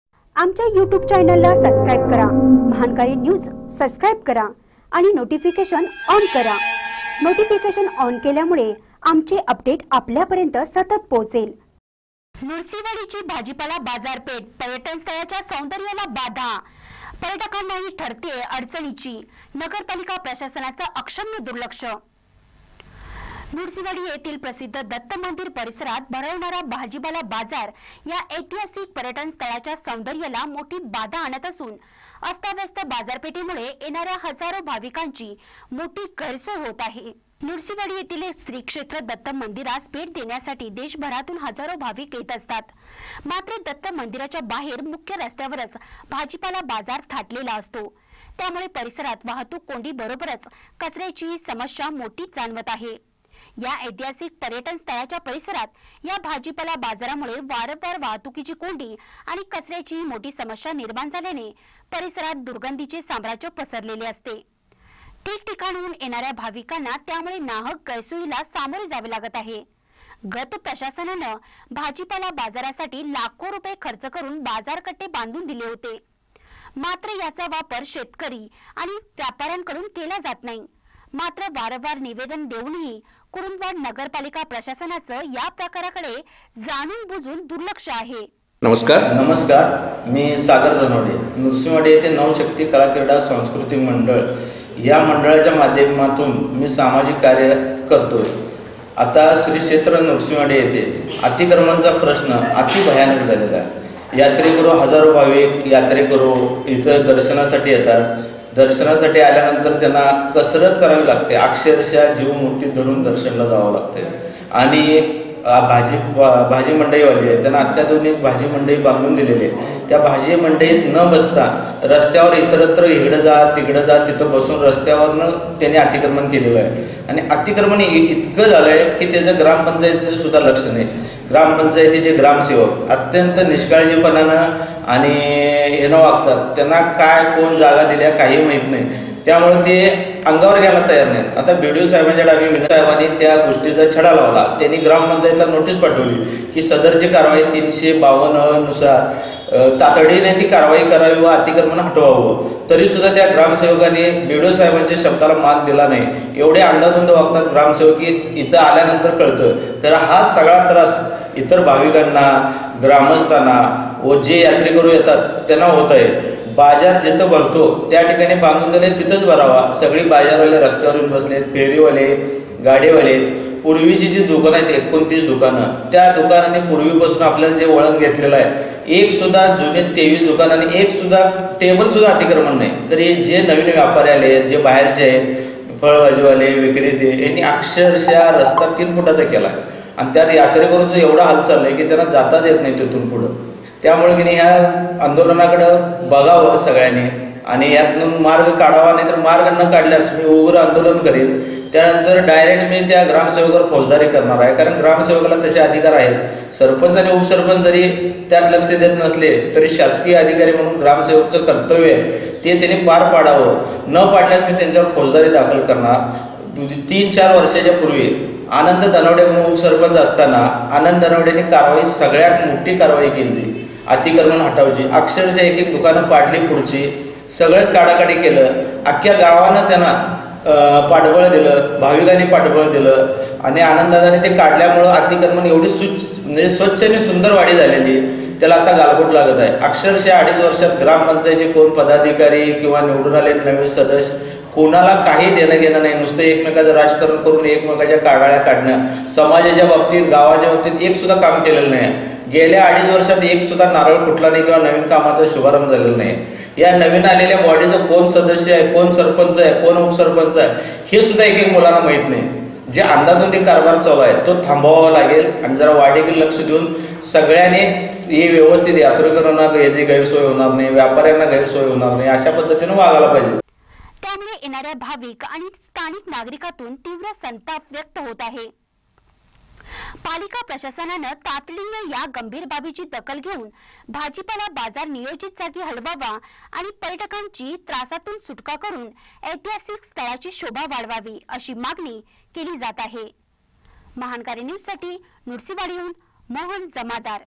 नृसिंहवाडीची भाजीपाला बाजारपेठ, पर्यटनस्थळाच्या सौंदर्याला बाधा : पर्यटकांनाही ठरतेय अडणचणीची
नृसिंहवाडीची भाजीपाला बाजारपेठ, पर्यटनस्थळाच्या सौंदर्याला बाधा : पर्यटकांनाही ठरतेय अडणचणीची ; नगरपालिका प्रशासनाचं अक्षम्य दुर्लक्ष